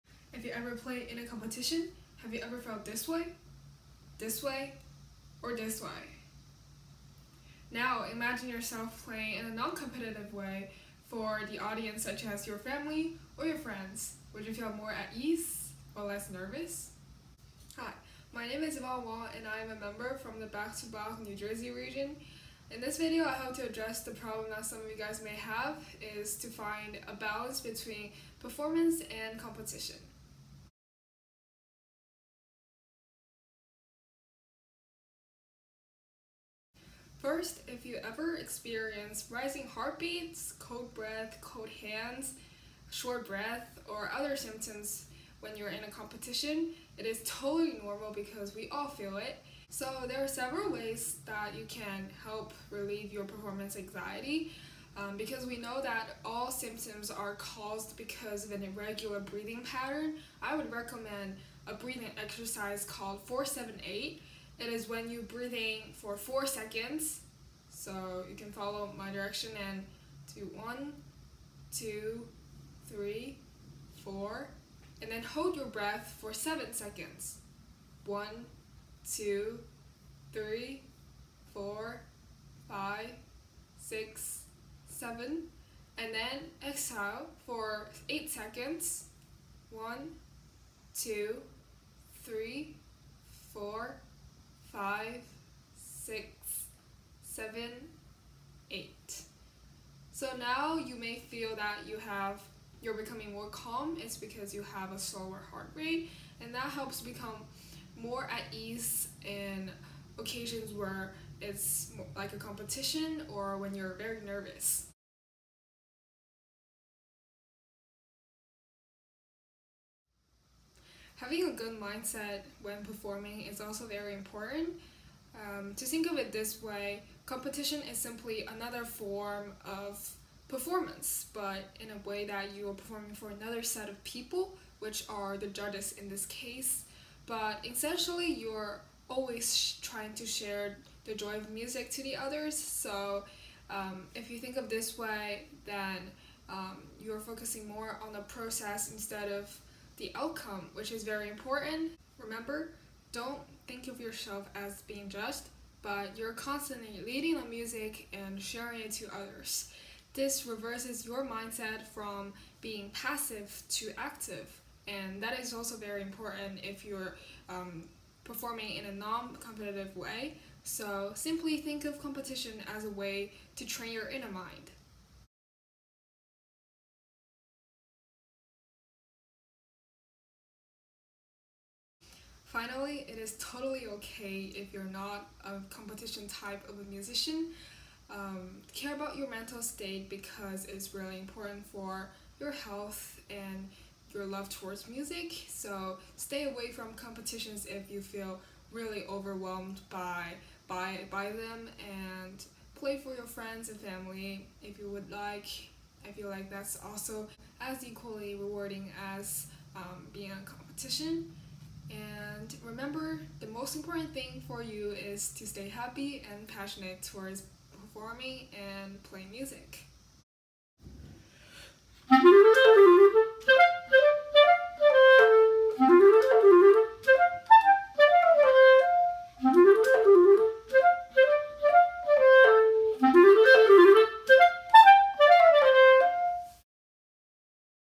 Back to Bach Urgent Care
Back to Bach Urgent Care: Walk-In Music Clinic
"Find a Balance Between Performance and Competition" with Clarinet Mentor, Yvonne Wang(BtB Mercer County NJ)